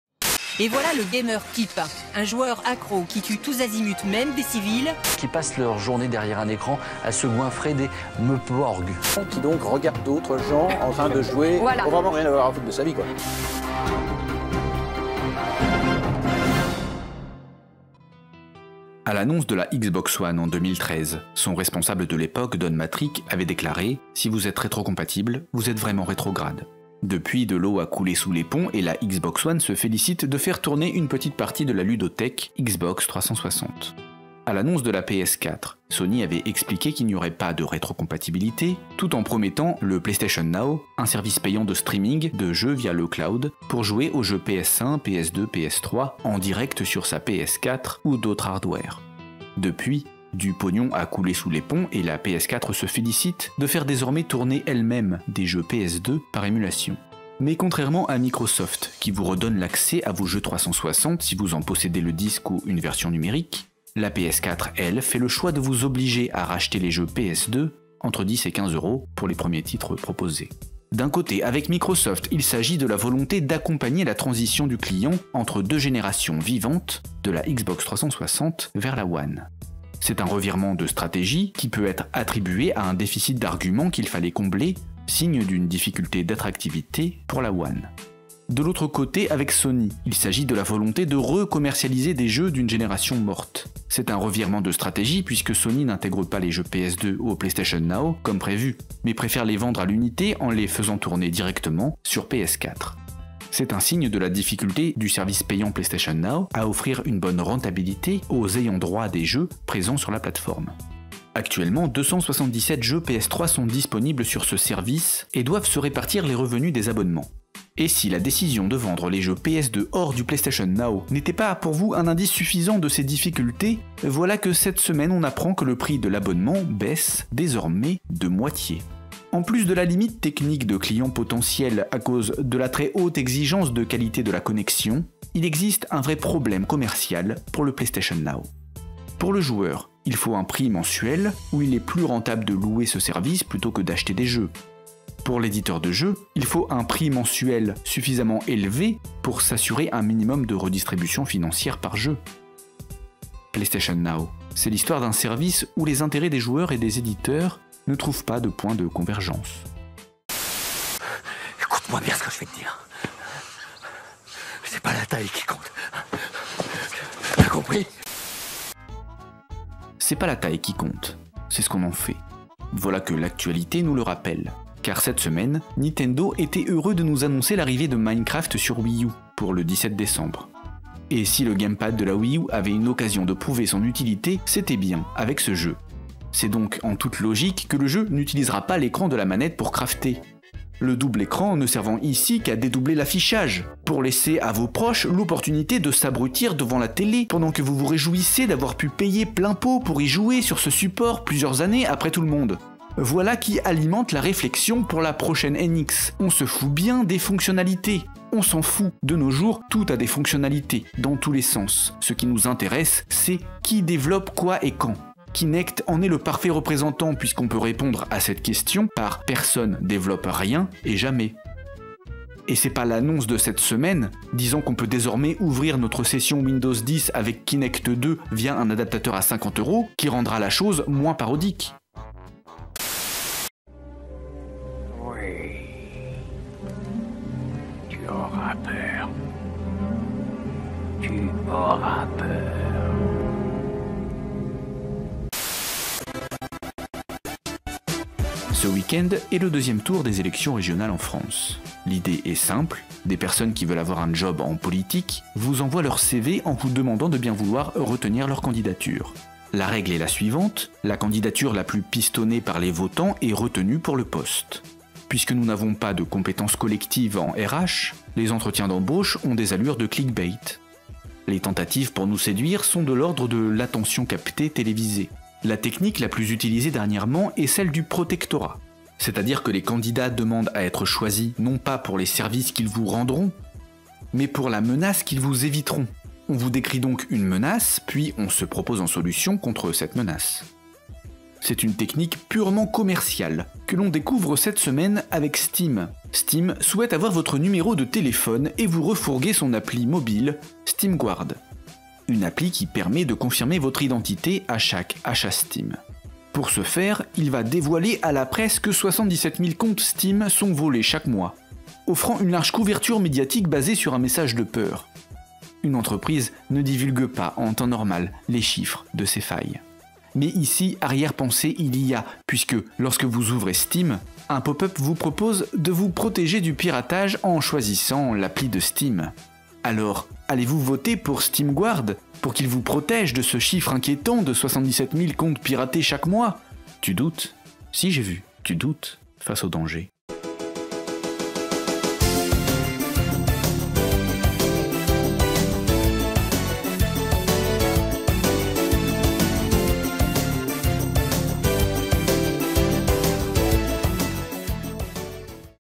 Free2Pay #56 : Intérêts non communs, fonctionnalités inutiles et Steam protectorat
Une semaine après avoir exposé le contenu sponsorisé de Fallout 4 sur le site des Inrockuptibles, analysé la stratégie de la conquête des écrans de Nintendo et expliqué le pourquoi du comment de la déclaration de Nicolas Sarkozy envers ces jeux d’une violence inouïe, l’équipe d’UnDropDansLaMare est de retour avec son épisode 56 de la chronique Free2Pay.

Et ce n’est pas car Noël approche qu’il était question de diminuer l’acidité de leur plume puisque cette semaine, c’est tout d’abord Sony, avec son PlayStation Now, qui reçoit une petite claque quant à ses difficultés à concilier les intérêts des joueurs et ceux des éditeurs... Puis c’est au tour de Minecraft dont la sortie approche, et qui rappelle, par la non-utilisation du Gamepad, que l’on se fout complètement des fameuses fonctionnalités exclusives des consoles... Et enfin, Steam sera le dernier de la liste avec sa manière bien à lui de « vendre » son application mobile afin de protéger son compte...